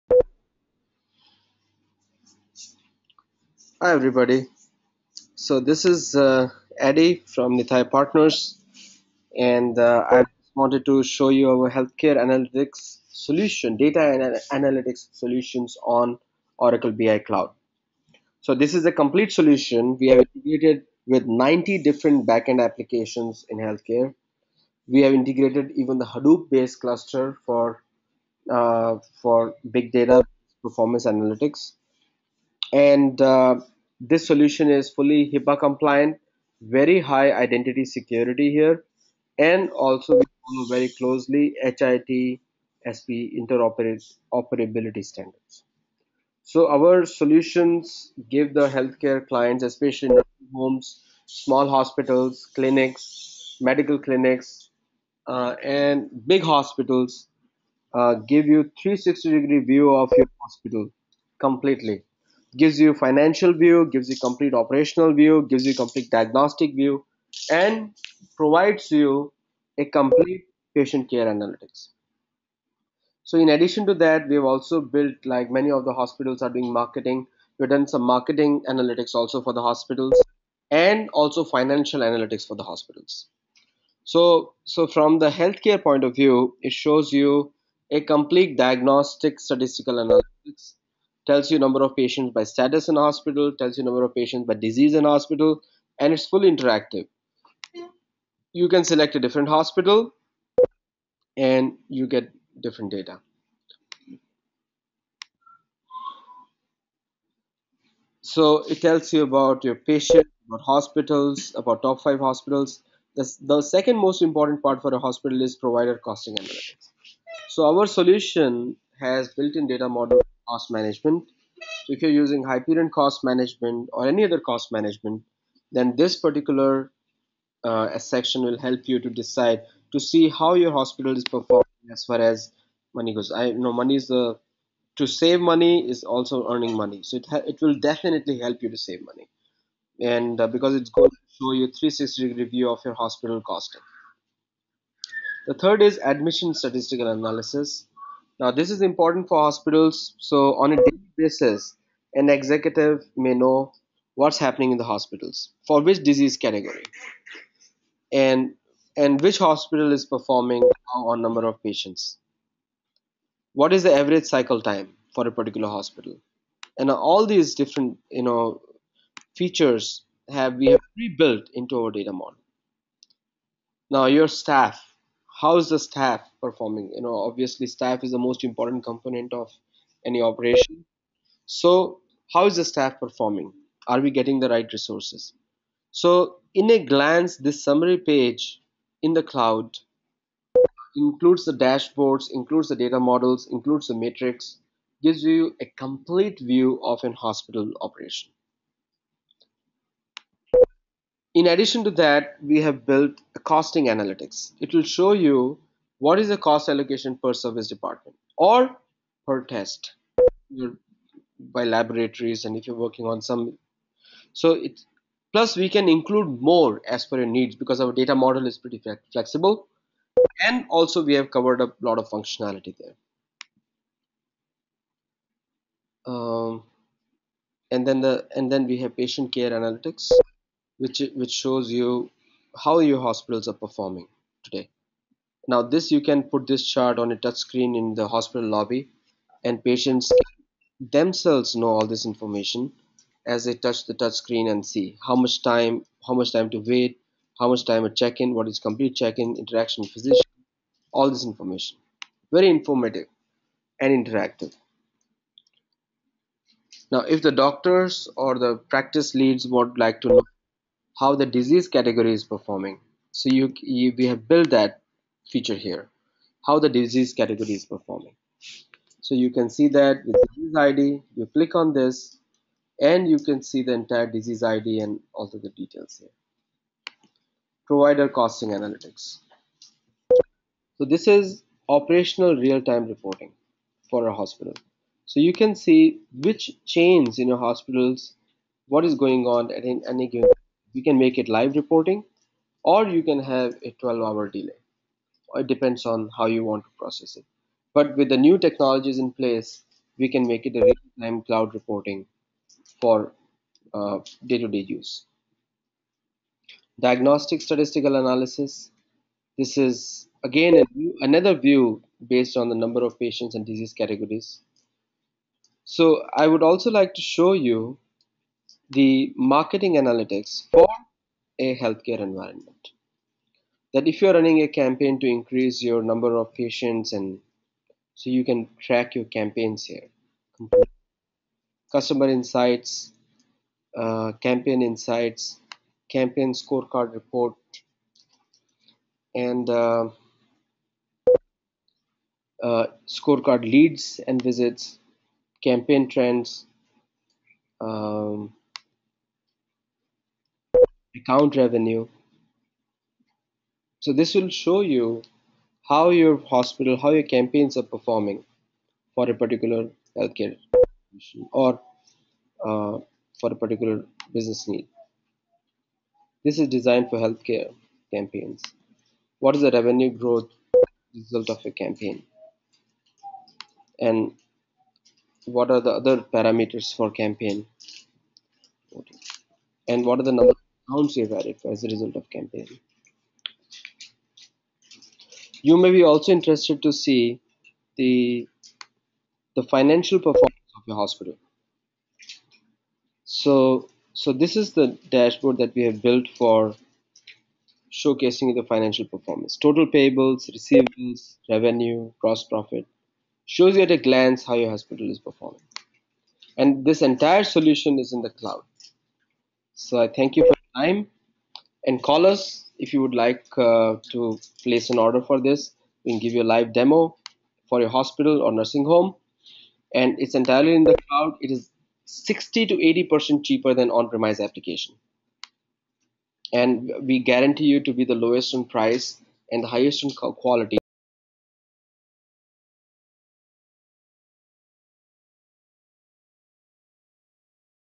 Healthcare Analytics Solution on Oracle BI Cloud
Nitai Partners build HIPAA compliant solutions for healthcare industry. Our solutions closely follow HITSP interoperability standards.

Our Oracle cloud based Data & Analytics platform gives you the following benefits 24X7:

    360 Degree view of your Hospital/Nursing-Home/Practice
    Complete Financial View
    Complete Operational View
    Complete Diagnostic View
    Patient Care Analytics

Our solutions on Oracle cloud platforms are almost 60% cheaper than a comparable on-Premise system.

Our solutions come pre-integrated with more than 90 healthcare applications using HIT (Healthcare IT) standards.

Healthcare Provider Analytics on Oracle BI Cloud platform include :

    Hospital Performance Analytics (16 Metrics to Measure hospital performance)
    Hospital Cost Management Analytics
    Patient Service Analytics
    Top 5 Hospitals (Based on Patient Visits)
    Cost Allocation
    Revenue
    Profitability
    Admission & Discharge Status
    Patient Cycle Time
    Cost Drivers (Medical Department, Service Department)
    Trend Metrics (MTD, YTD)
    Healthcare Marketing Analytics

We integrate your healthcare systems data in a matter of weeks.